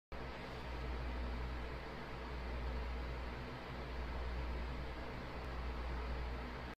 This is the real RiD Bludgeon
This minicon is the real Bludgeon of Transformers Robots in disguise.